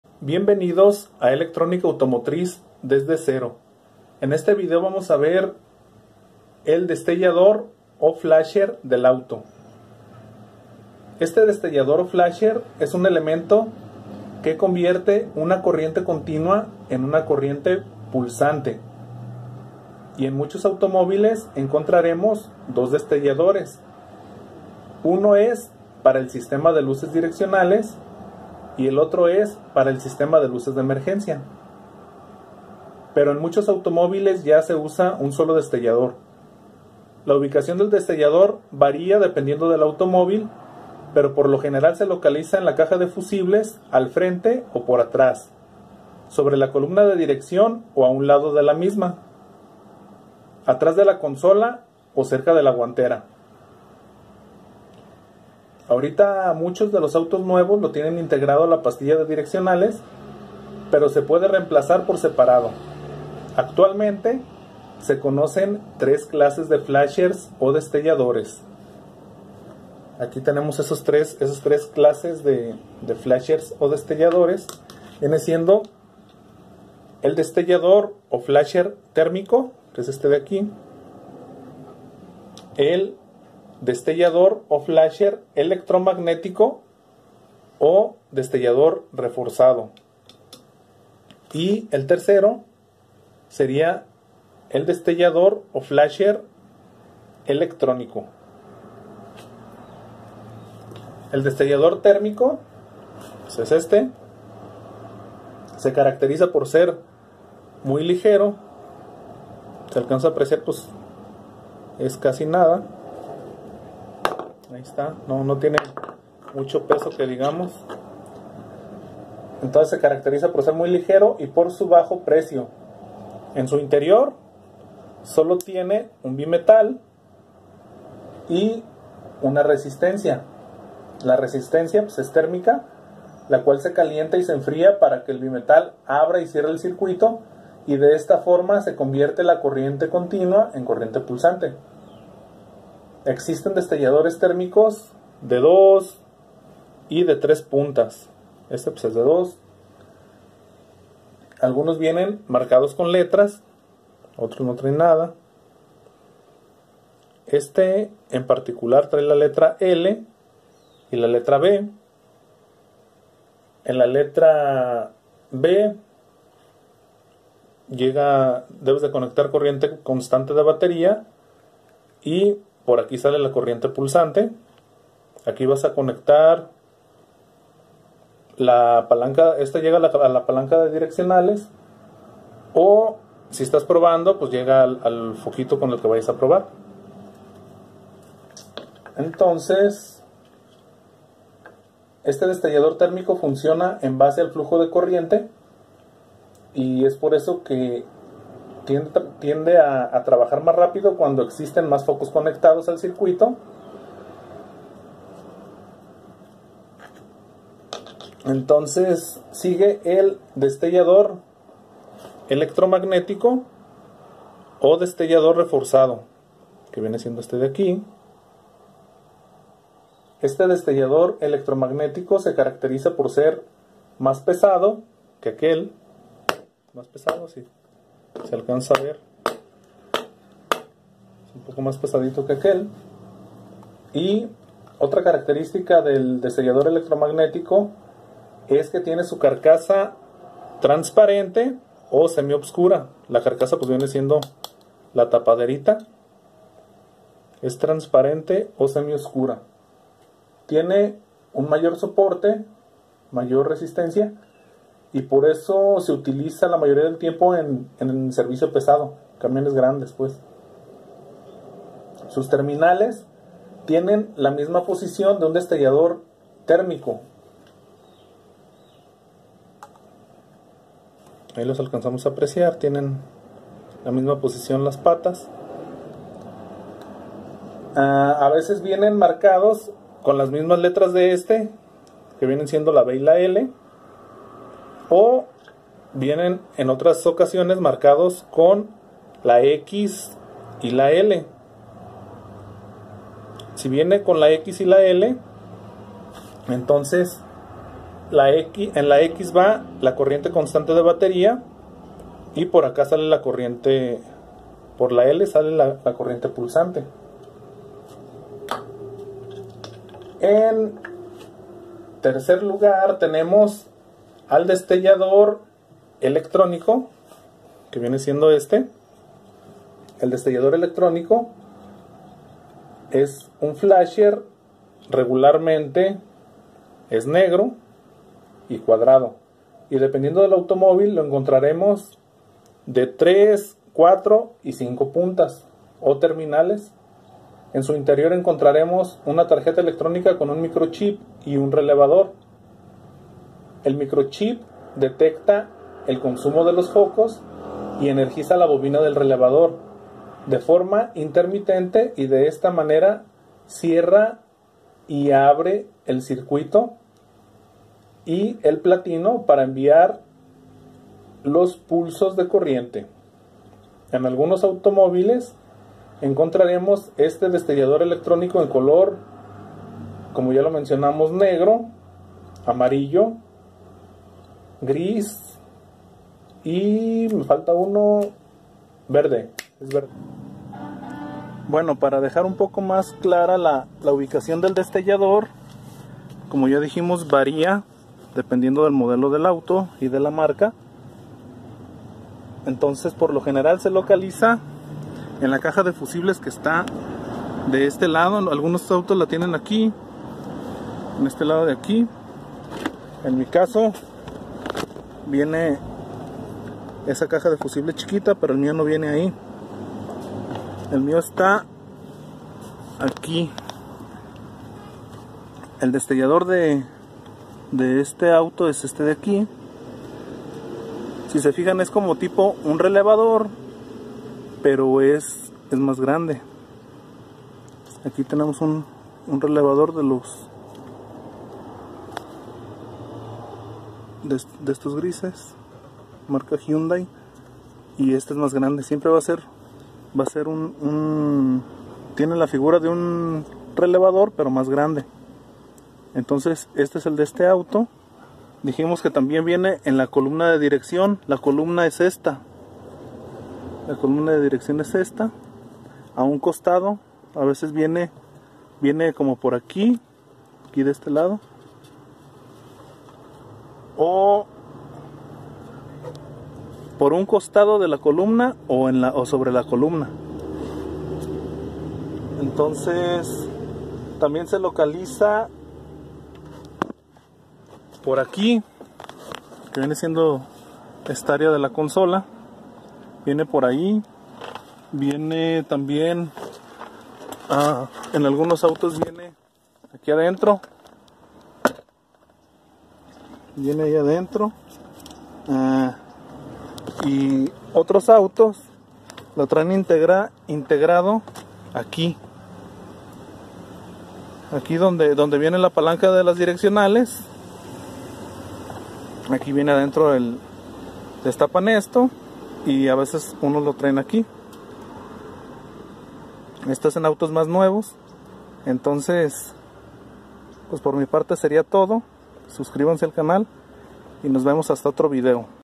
QUE ES UN DESTELLADOR O FLASHER DEL AUTO ?
en este video te enseño que es un DESTELLADOR y que es un flasher